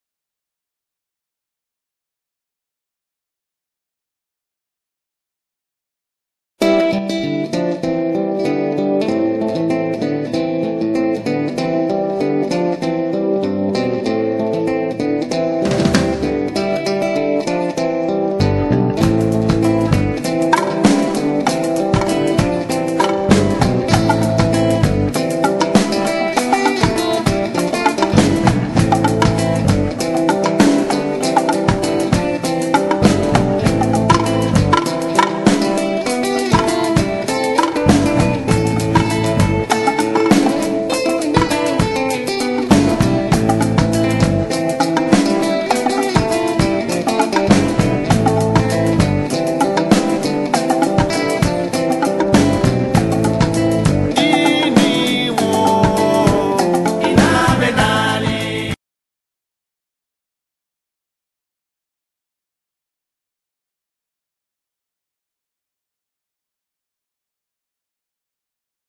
Blue pourer pouring wine
Auper Eclipse ID dispensing wine by the glass demo.